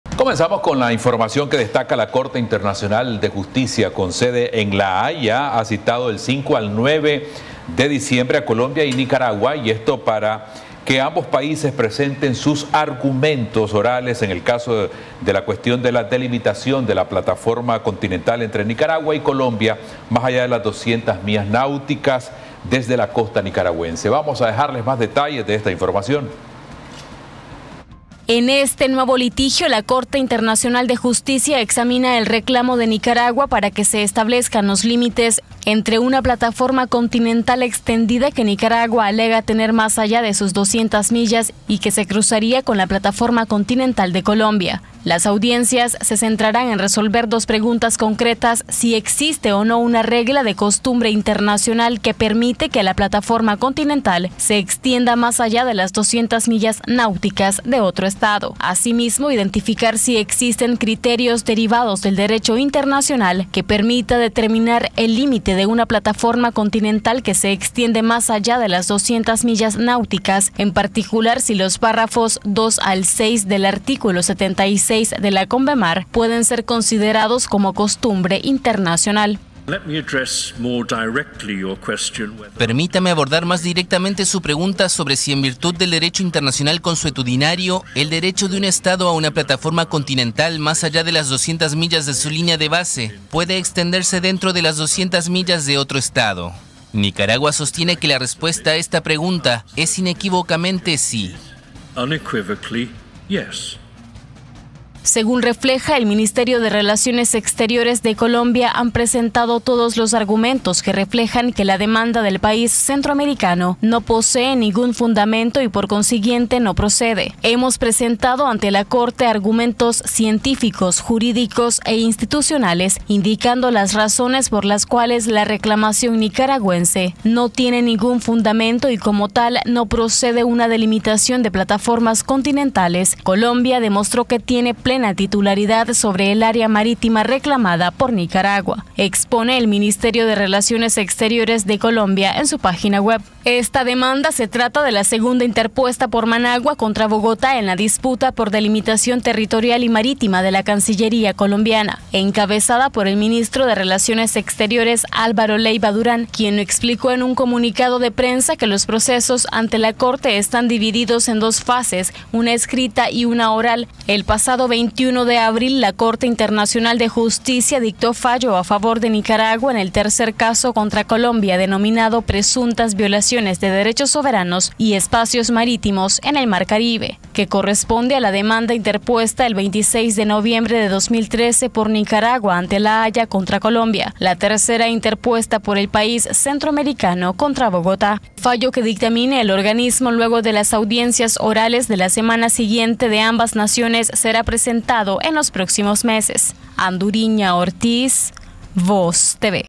Nicaragua y Colombia acudirán a la CIJ de la Haya para audiencias orales
La Corte Internacional de Justicia (CIJ), con sede en La Haya, ha citado del 5 al 9 de diciembre a Colombia y Nicaragua para que ambos países presenten sus argumentos orales en el caso “Cuestión de la delimitación de la plataforma continental entre Nicaragua y Colombia más allá de las 200 millas náuticas desde la costa nicaragüense (Nicaragua c. Colombia)”.